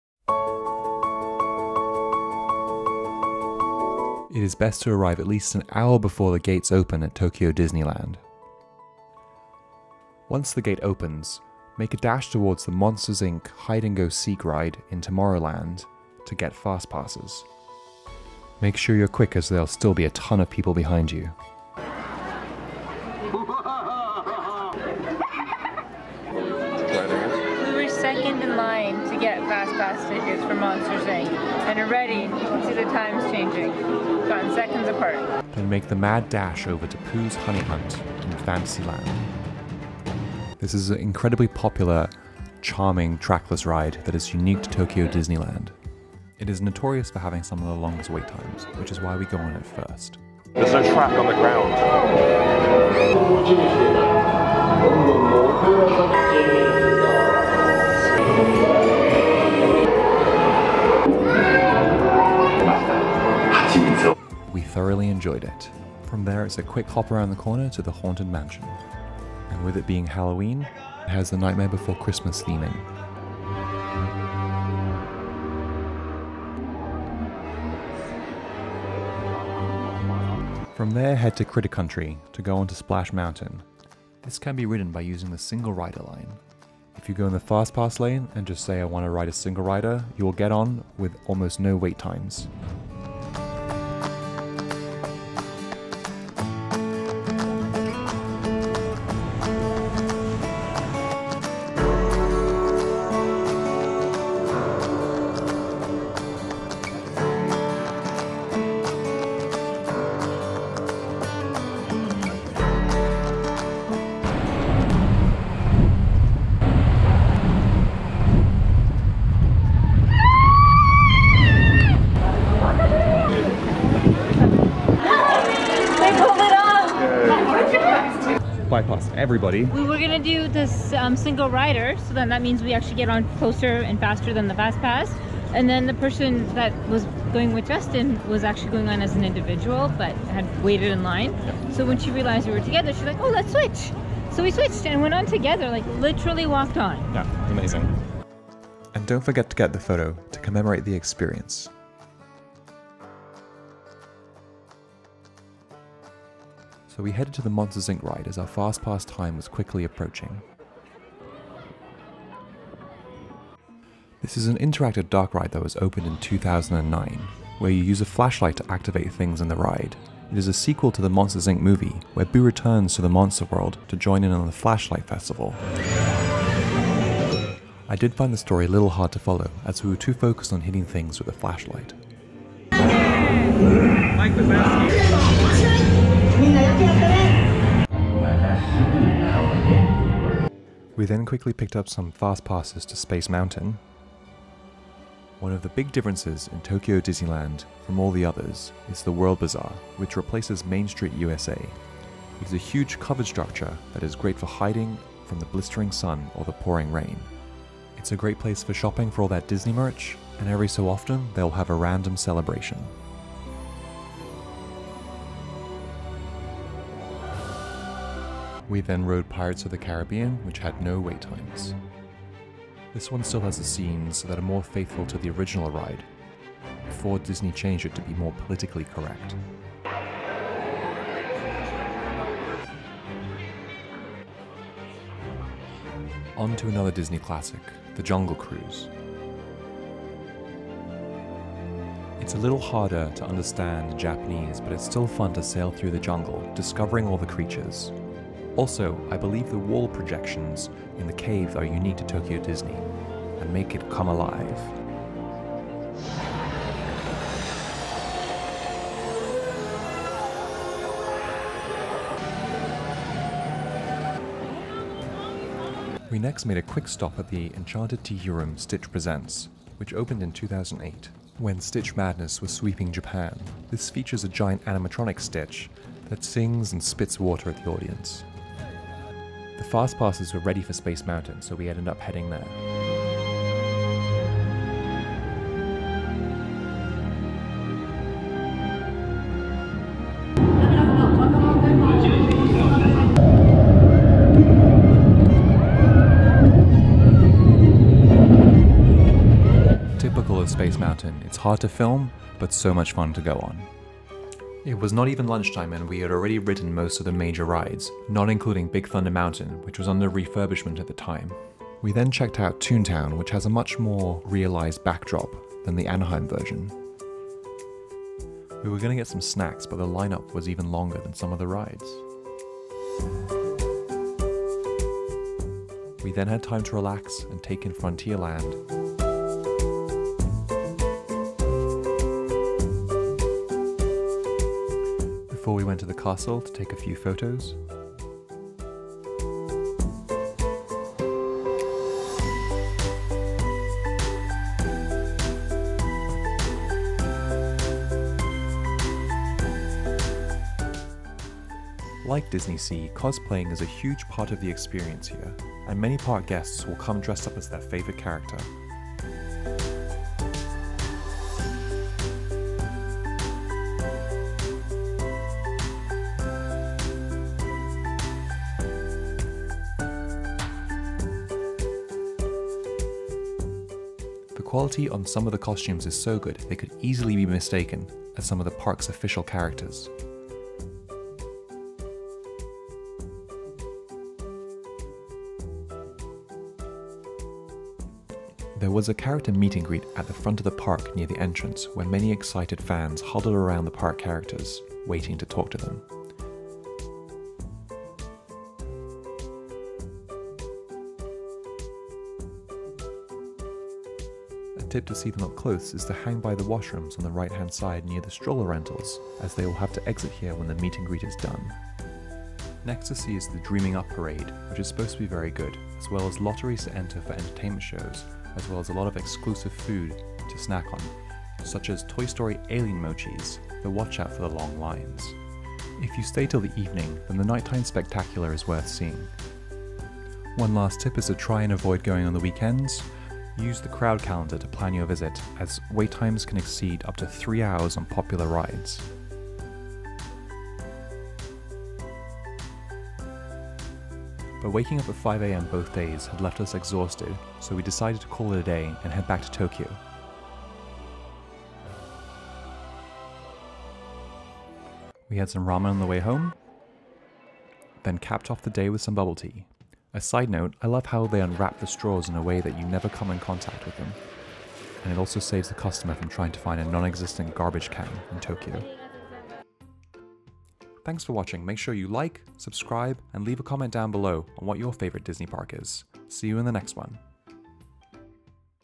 Tokyo Disneyland 1 day Itinerary - watch before you go 東京ディズニーランド
Follow these steps  to optimize your day at Tokyo Disneyland

For crowd calendar check out but you will need a translator like Chrome to help.

DSLR Camera

Street Photography Camera

Point and Shoot Camera

Zoom Lens

Prime Lens